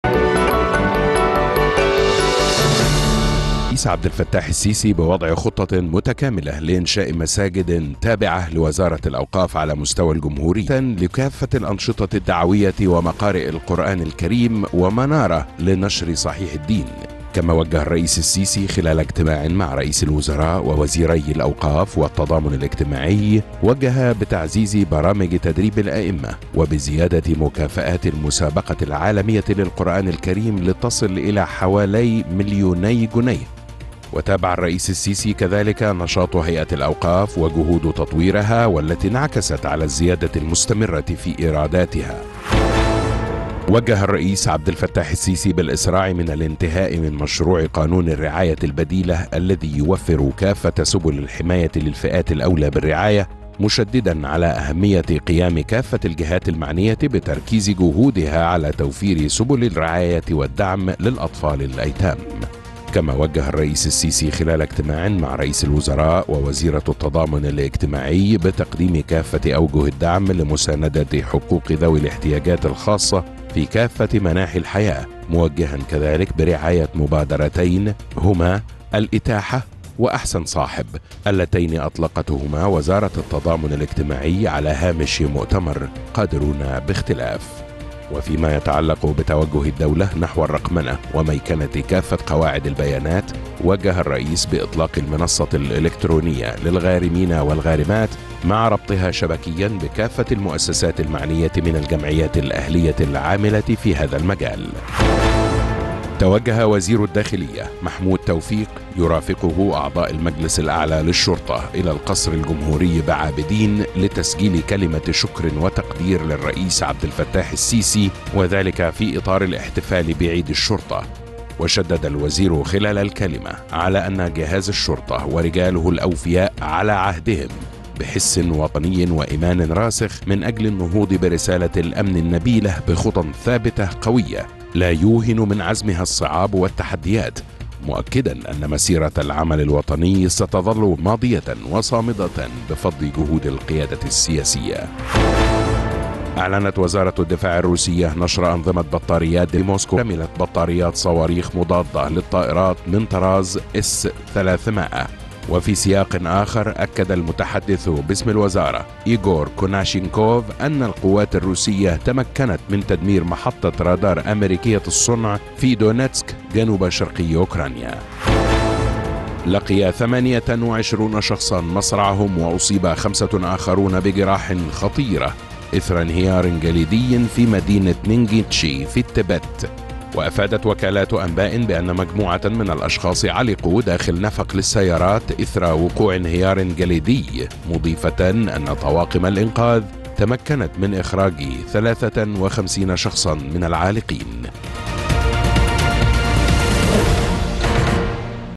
موجز أخبار السابعة مساءً - الرئيس السيسي يوجه بسرعة الإنتهاء من مشروع قانون الرعاية البديلة
أخبار أون